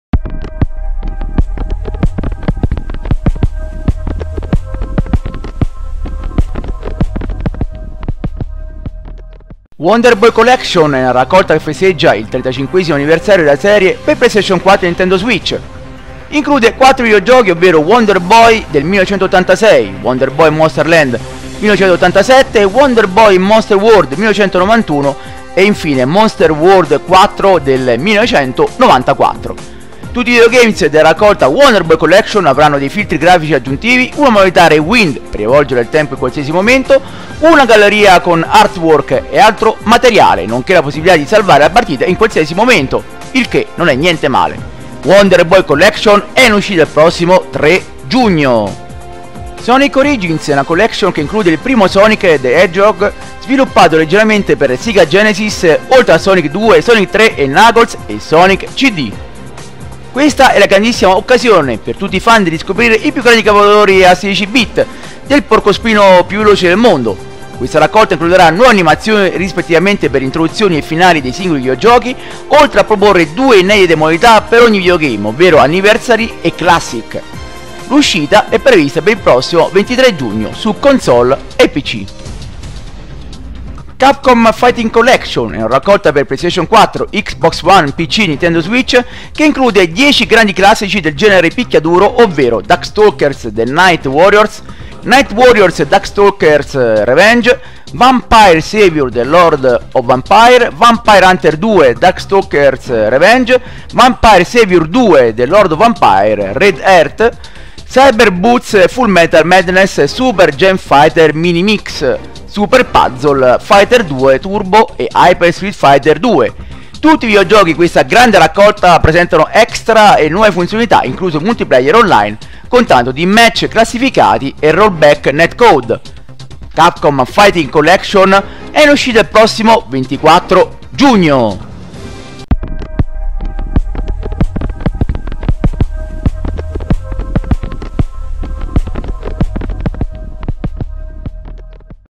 GGN | Giugno 2022
★ Vi ringrazio per i Likes e per le condivisioni ★

Konnichiwa GiappoLovers !!!
Eccoci arrivati al primo giorno del mese di giugno2022 e quindi è giunto per noi il momento del classico video GAMING NEWS per quanto riguarda le migliori uscite dei videogames giapponesi qui in Italia!!! Buona visione a tutti Voi yeahhhhh yattaaaaaaa ^_^